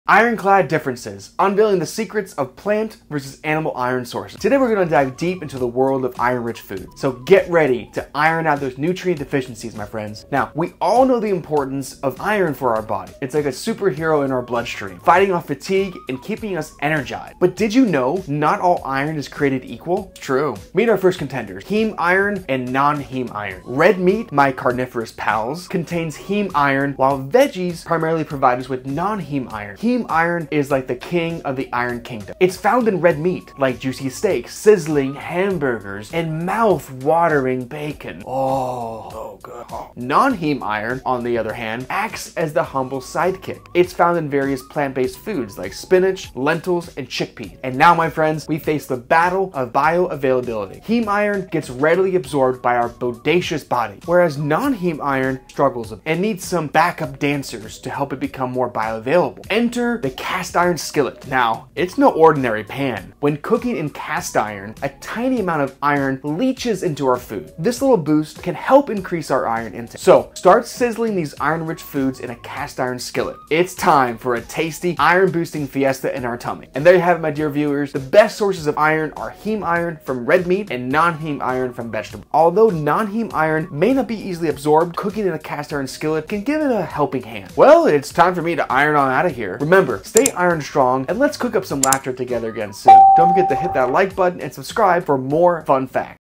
Plant vs Animal Iron Sources: Unveiling the Key Differences
In this insightful video, we reveal the secret differences between plant and animal sources of iron - an essential mineral for our overall well-being.

Did you know that there are significant variations in how our bodies absorb iron from plant-based and animal-based foods? With expert guidance, we uncover the intricate details behind these differences, helping you make informed dietary choices.

Join us as we explore the bioavailability of iron in plants and animals, highlighting their contrasting absorption rates and factors influencing them. Discover how the presence of certain compounds like heme iron affects our iron intake and utilization. Additionally, we discuss the impact of diverse diets, such as vegan, vegetarian, and omnivorous, on iron levels in the body.

Our team of experienced nutritionists and researchers ensures this video is packed with evidence-based information and scientific explanations, guaranteeing you a comprehensive understanding of the subject.

Empower yourself with knowledge and take control of your well-being!